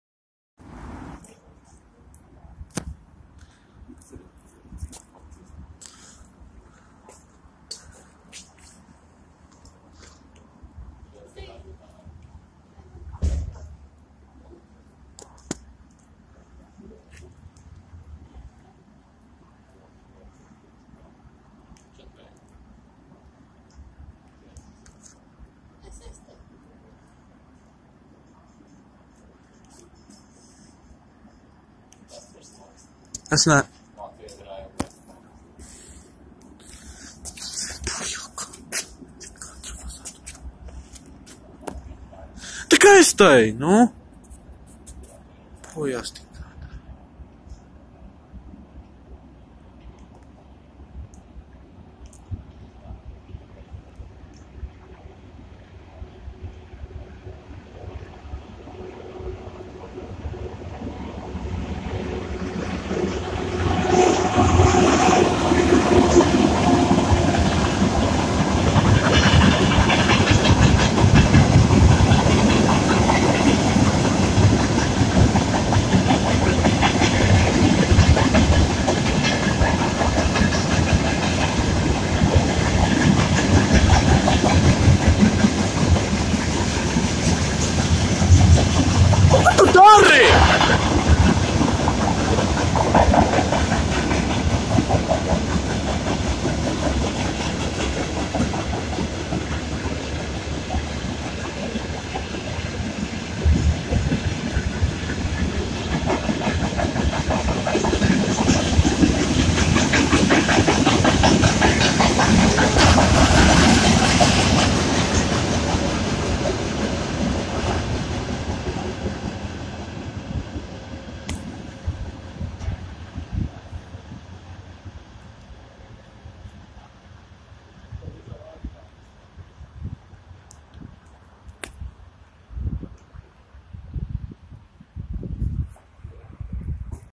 2M62UM-0094 (LDZ Cargo) with mixed freight train passing station Ogre
Ogre, Latvia
5/11/2018
17:12